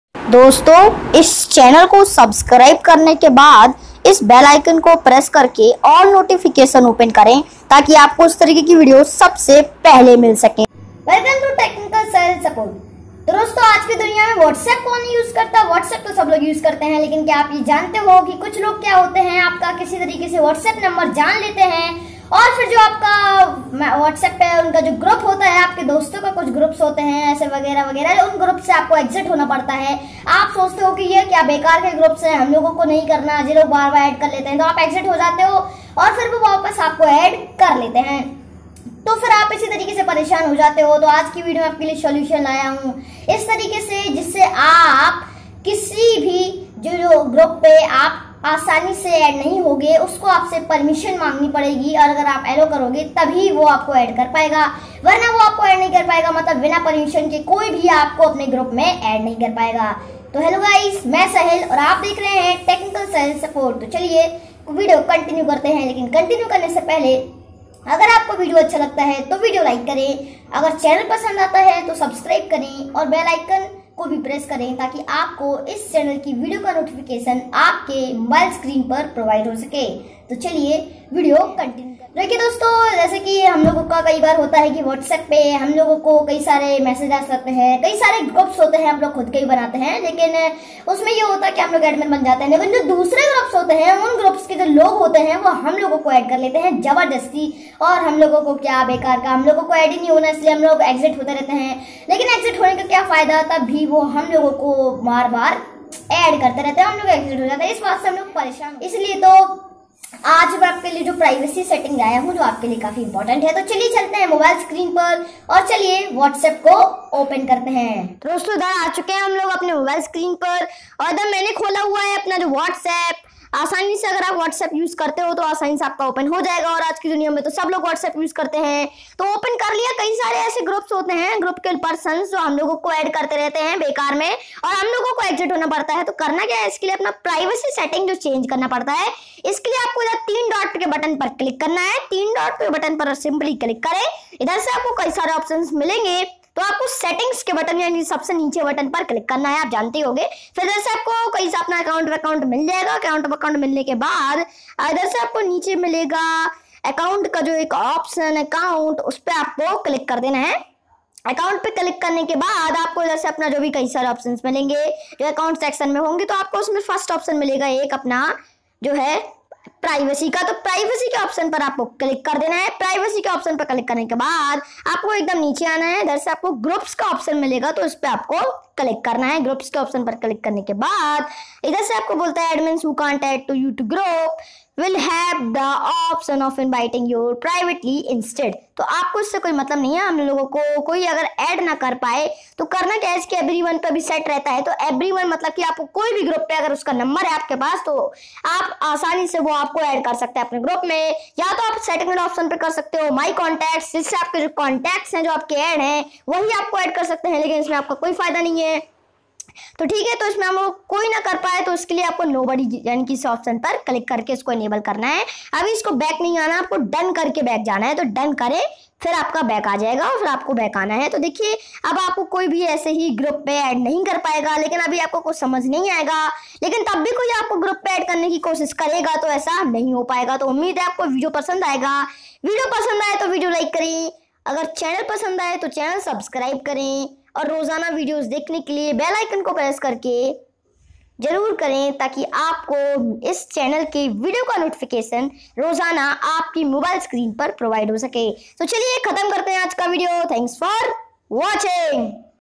WhatsApp group privacy settings | Choose who can add me to the group | Everyone,My Contacts, Nobody
Hi guys
I am Sahil and you are watching TECHNICAL SAHIL SUPPORT.

Dosto aaj ki video mein Maine WhatsApp group privacy settings ke bare mein bataya hai jisse koi Bina permission liye aapko add Nahi Kar sakta hai.

Ummeed hai aapko video Pasand aayega.